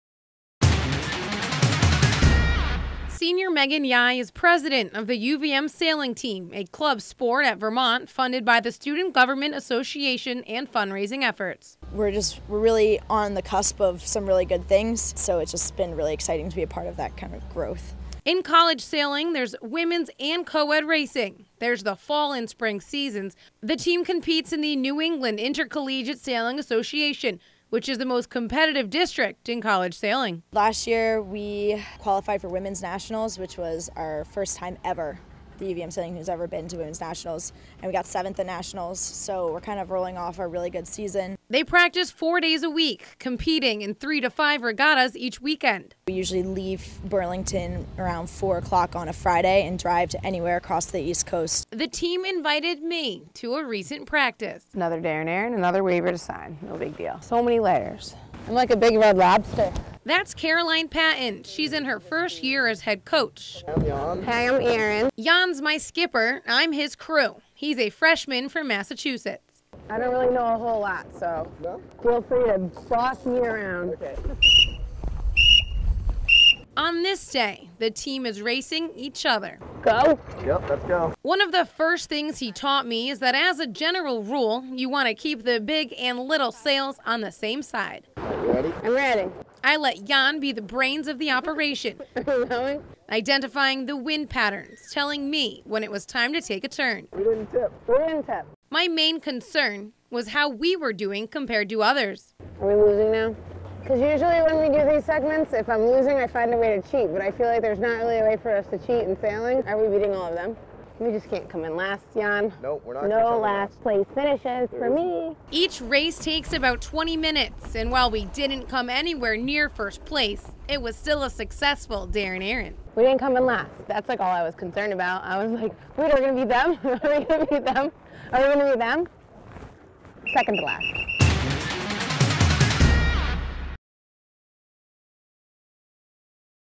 Darin' Erin: UVM Sailing Team
Erin joins the UVM Sailing Team for a recent practice on Lake Champlain